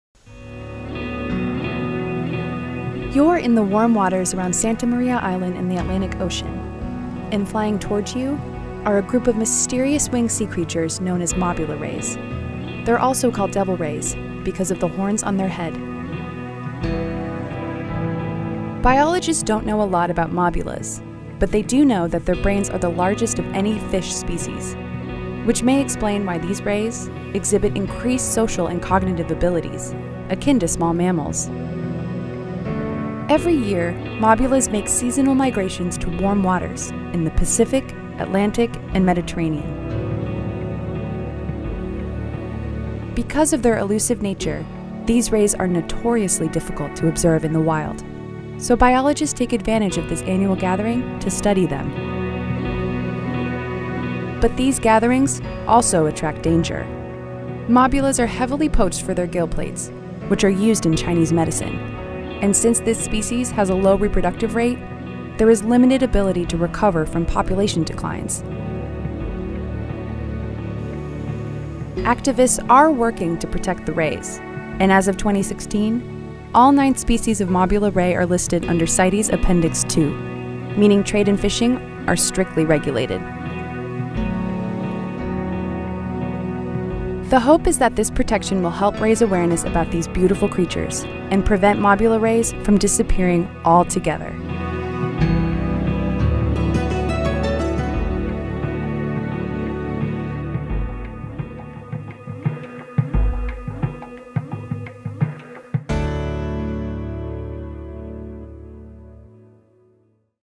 Diving Underwater With Elusive Devil Rays
Devil rays are mysterious sea creatures that are rarely seen in the wild. With their population dwindling, biologists are racing to learn more about the species before devil rays disappear altogether.